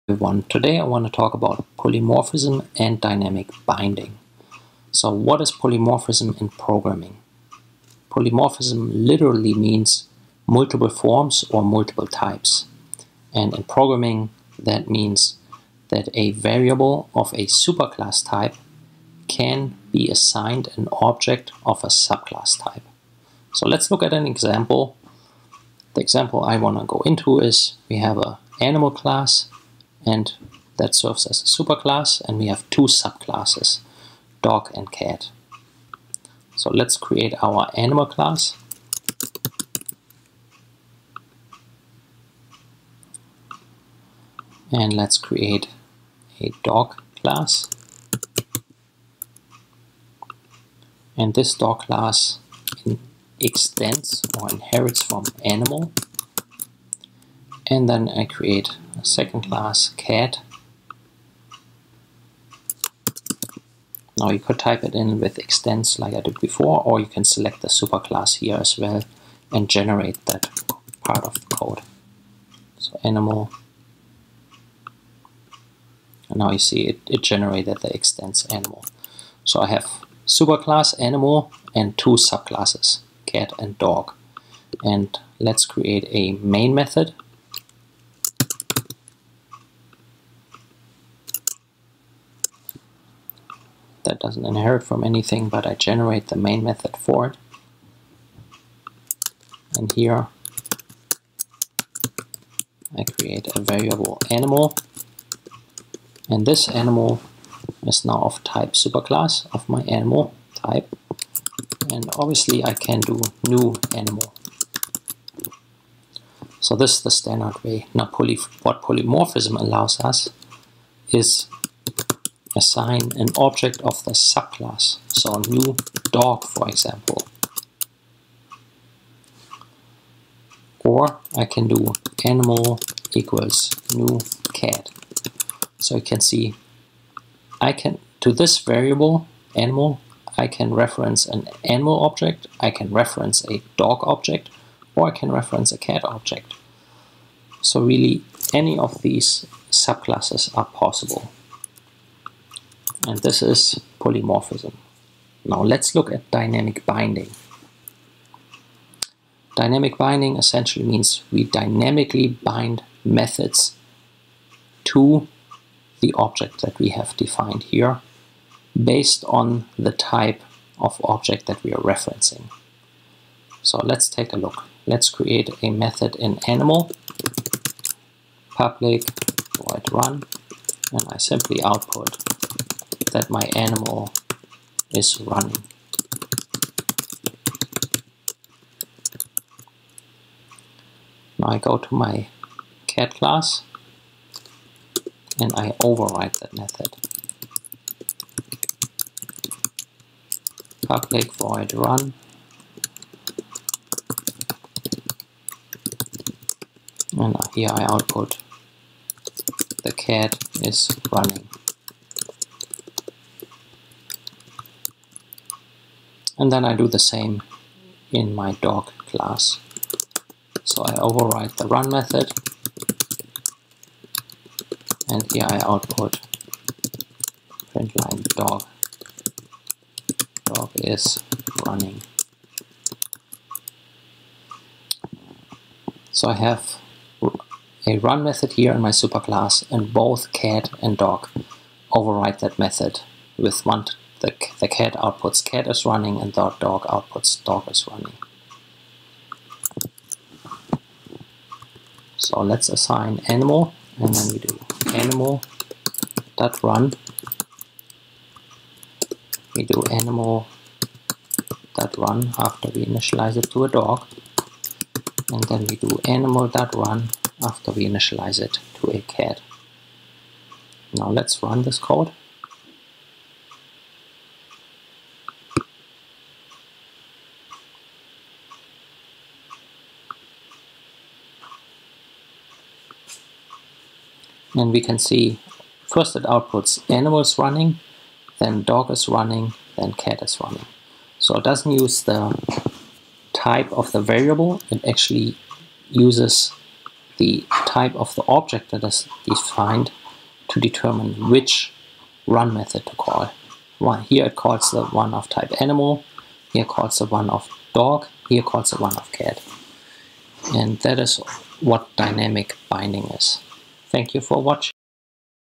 Java Tutorial: Polymorphism and Dynamic Binding
This video explains polymorphism and dynamic binding.